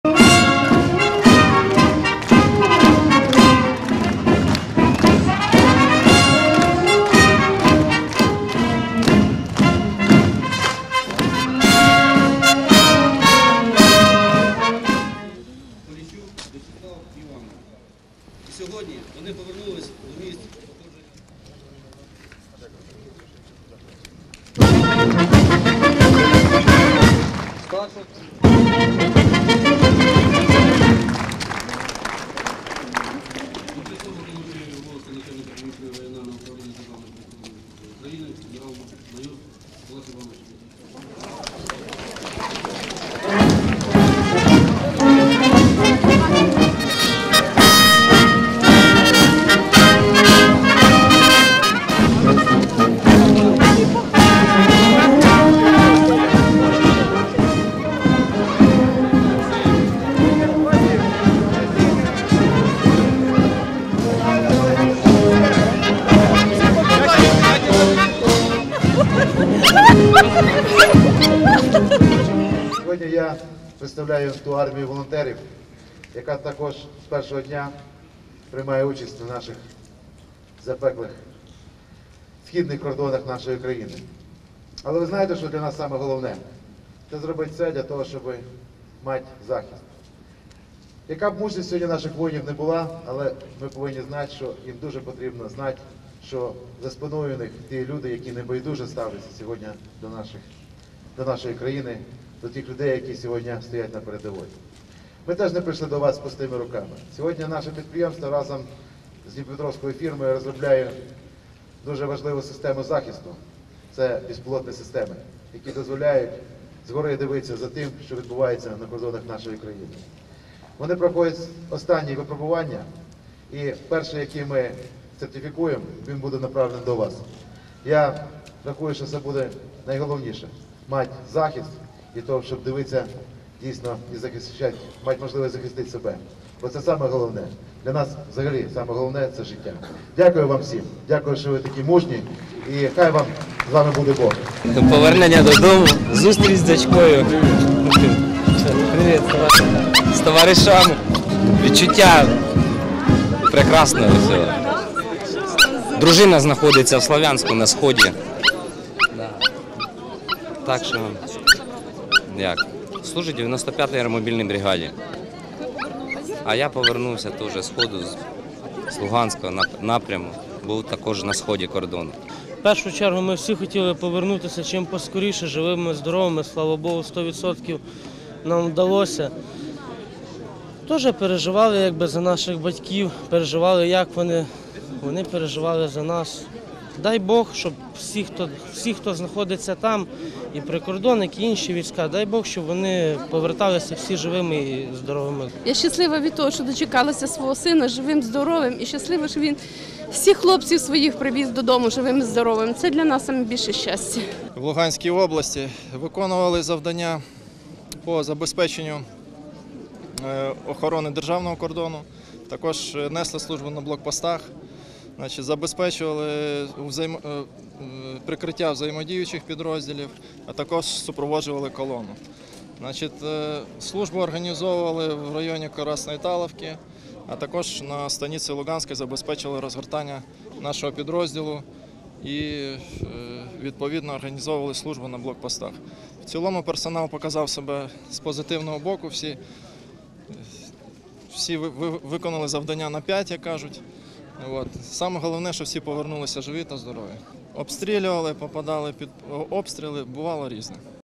Житомирські прикордонники, які 2,5 місяці несли службу в АТО, повернулися додому - Житомир.info
54 прикордонника повернулися у Житомир із зони АТО. З початку серпня вони виконували завдання на Луганщині, охороняючи кордон з Росією. Прикордонники не приховують: не раз потрапляли під обстріли, втім радіють, що повернулися без втрат.
Десятки людей з квітами і синьо-жовтими прапорами прийшли зустрічати своїх рідних, яких не бачили два з половиною місяці

Житомирские пограничники, 2,5 месяца несли службу в АТО, вернулись домой - Житомир.info